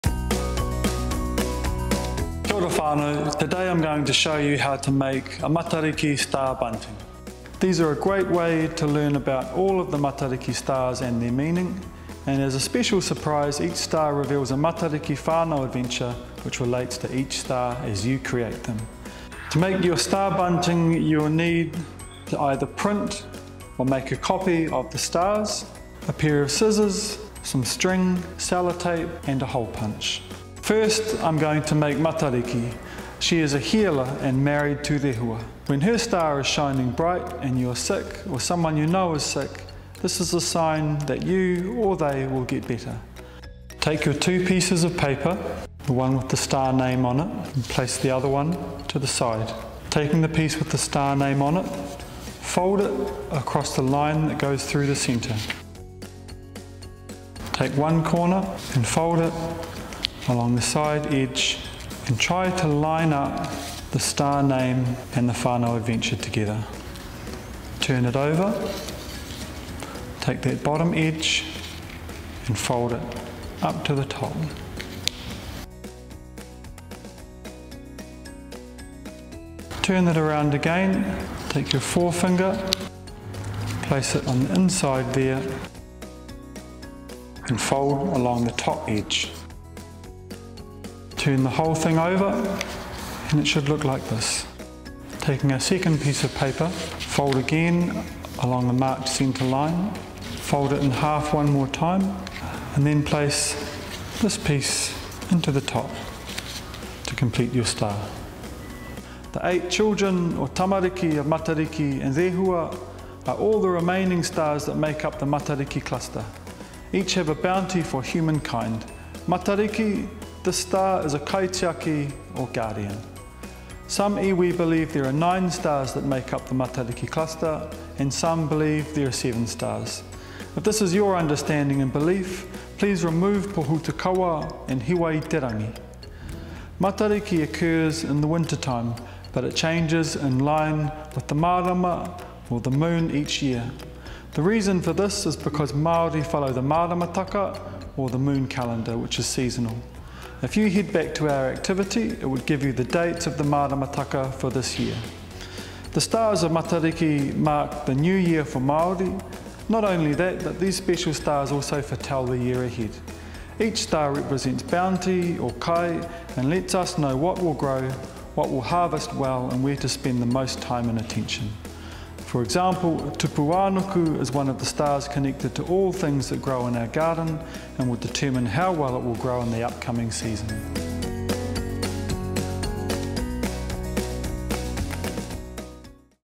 Matariki Star Bunting
In this activity tamariki make nine stars that form the Matariki constellation, learn their names, and each star's significance.

For each star there is also a specific whānau adventure which appears when the star is made.  For example the Matariki star itself is the guardian of the other stars in the cluster. She is a healer and when her star is shining bright and you're sick or someone you know is sick, it is a sign the person will get better. The whānau adventure for this star is to show aroha to someone you know who is having a challenging time.